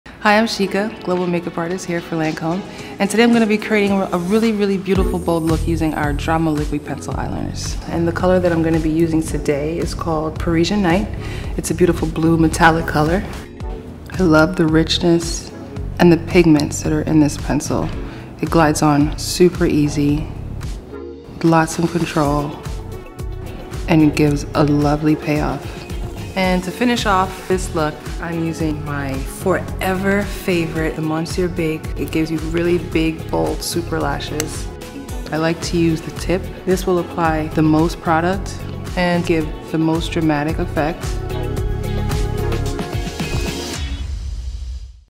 HOW TO DO A BLUE EYE MAKEUP LOOK | LANCÔME
Get ready to shine with this stunning blue metallic eye makeup look! Watch Lancôme International Makeup Artist, Sheika Daley show you how to create a bold eye look using the Lancôme Drama Liqui-Pencil Eyeliner. For the finished look, Sheika applies the Monseiur Big Mascara for dramatic, voluminous lashes.